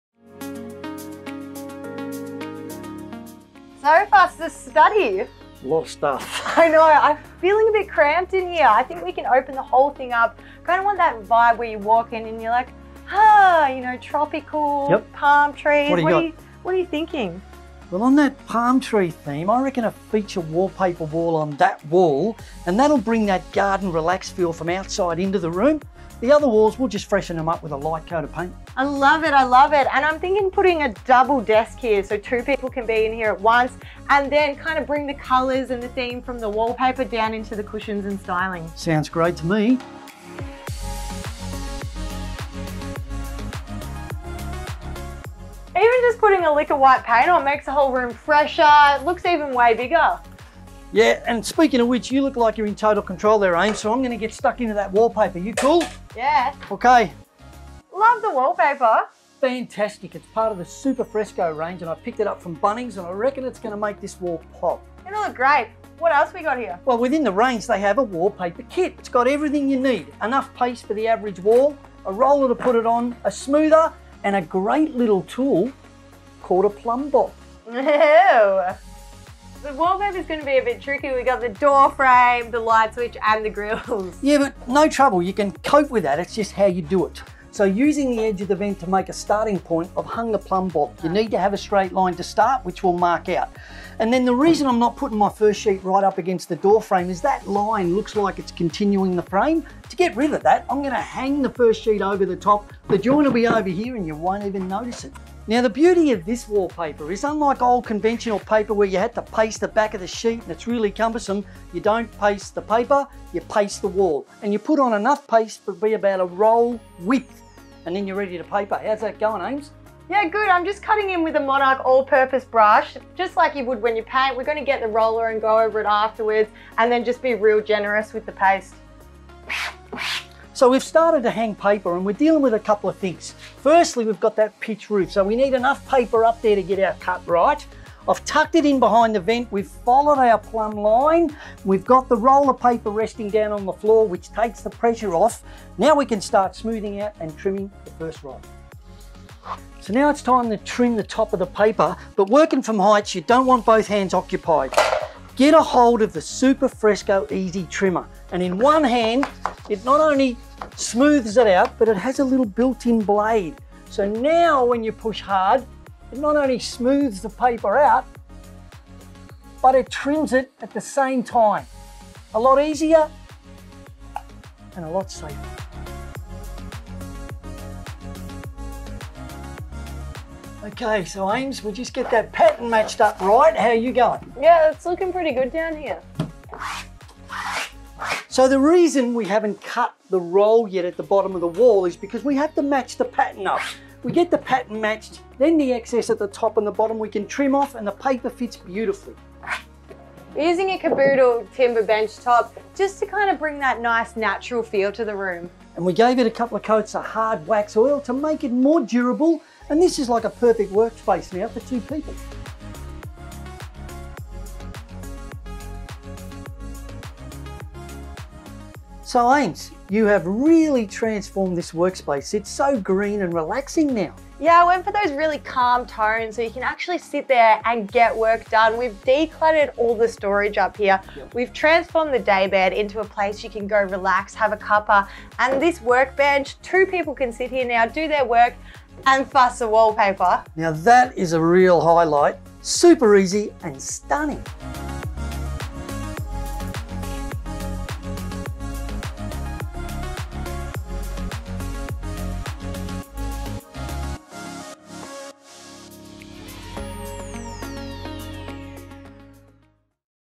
HoM-Transform outdated study into a tropical paradise 🌿 👉
🔨 Attention DIY'ers! 👀 House of Monarch's Study Transformation is now available! 📺🏡 👀 Watch as Fussy and Aimee using some amazing wallpaper by Superfresco, a sturdy, timber benchtop from Kaboodle and some on-point styling to transform the study room. 🌿 Learn how you too can update your study room with helpful tips and expert advice by tuning in! 🙌 👉